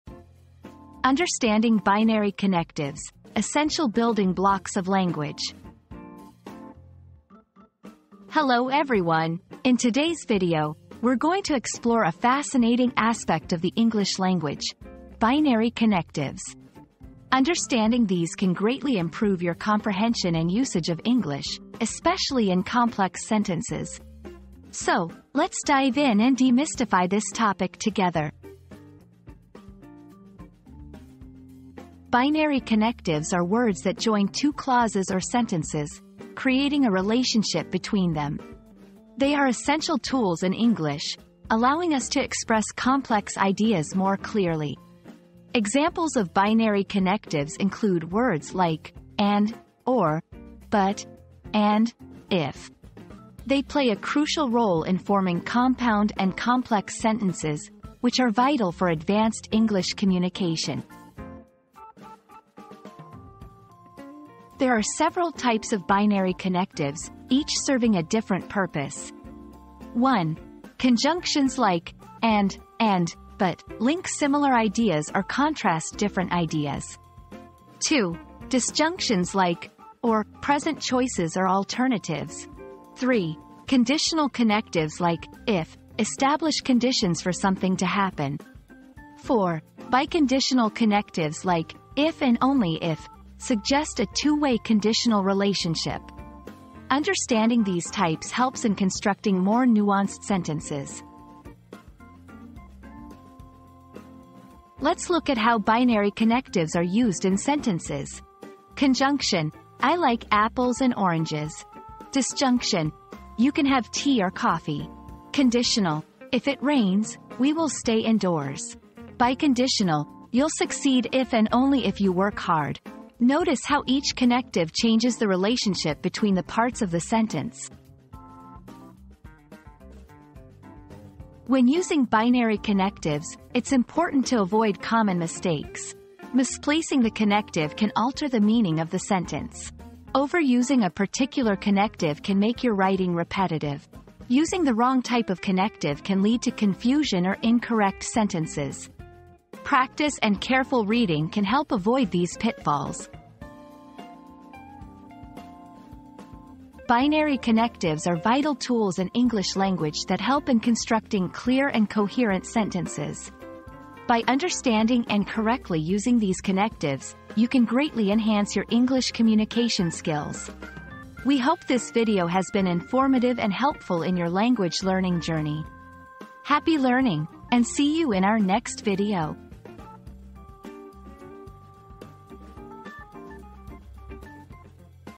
Understanding Binary Connectives: Essential Building Blocks of Language
Cracking the Code: Understanding Binary Connectives • Unlock the secrets of language with this captivating video that dives deep into the essential building blocks known as binary connectives. Discover how these powerful tools shape our understanding and communication.

00:00 • Introduction - Understanding Binary Connectives: Essential Building Blocks of Language
00:31 • What are Binary Connectives?
01:04 • Types of Binary Connectives
01:44 • Using Binary Connectives in Sentences
02:10 • Common Mistakes to Avoid